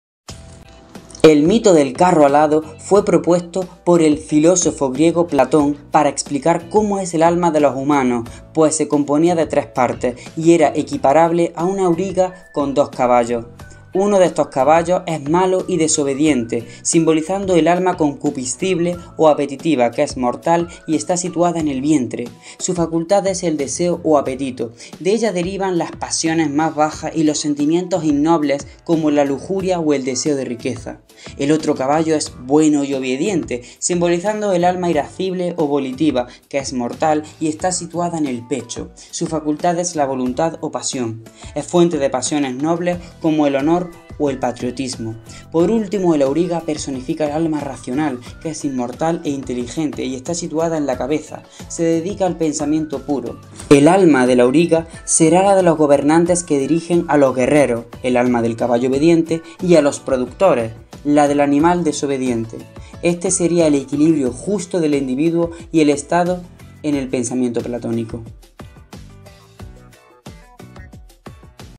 ¿Qué es el MITO DEL CARRO ALADO de PLATÓN?
El mito del carro alado fue propuesto por el filósofo griego Platón para explicar cómo es el alma de los humanos, pues se componía de tres partes y era equiparable a un auriga con dos caballos. Uno de estos caballos es malo y desobediente, simbolizando el alma concupiscible o apetitiva, que es mortal y está situada en el vientre. Su facultad es el deseo o apetito. De ella derivan las pasiones más bajas y los sentimientos innobles como la lujuria o el deseo de riqueza. El otro caballo es bueno y obediente, simbolizando el alma irascible o volitiva, que es mortal y está situada en el pecho. Su facultad es la voluntad o pasión. Es fuente de pasiones nobles como el honor o el patriotismo. Por último, el auriga personifica el alma racional, que es inmortal e inteligente y está situada en la cabeza. Se dedica al pensamiento puro. El alma del auriga será la de los gobernantes que dirigen a los guerreros, el alma del caballo obediente, y a los productores, la del animal desobediente. Este sería el equilibrio justo del individuo y el estado en el pensamiento platónico.